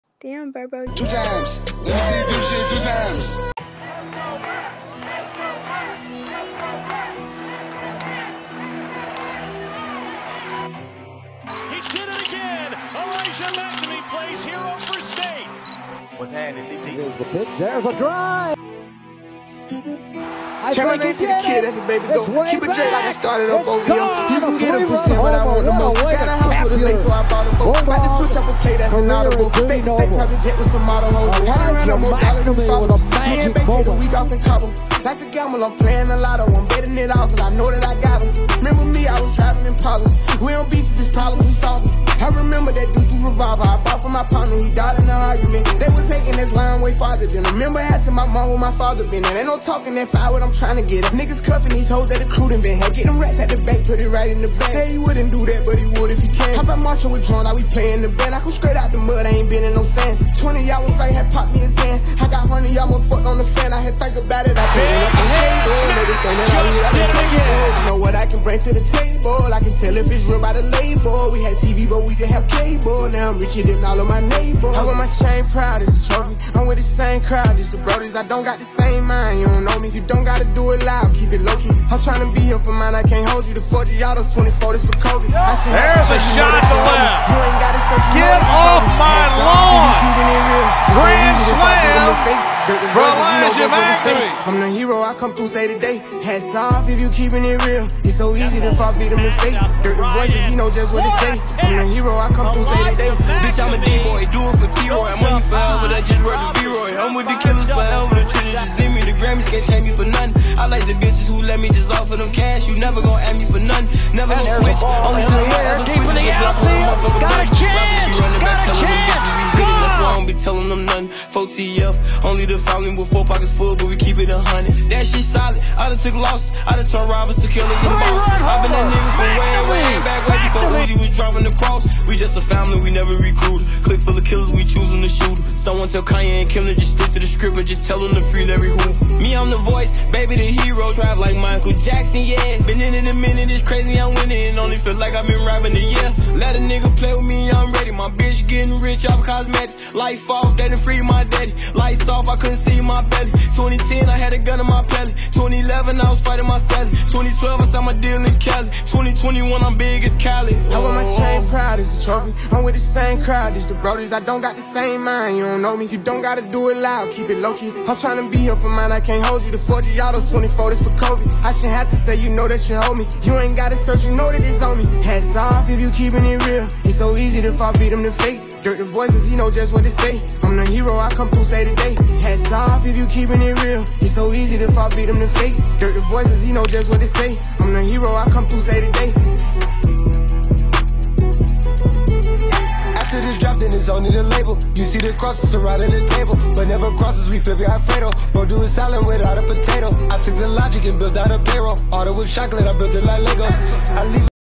Elijah MacNamee Career Highlights⚾️ “Big Hit Mac”
Big S/O to the bro Elijah.. I asked him if I could make a tape him, He gave me the song and the rest was history

Follow me and I’ll follow back 💪🏽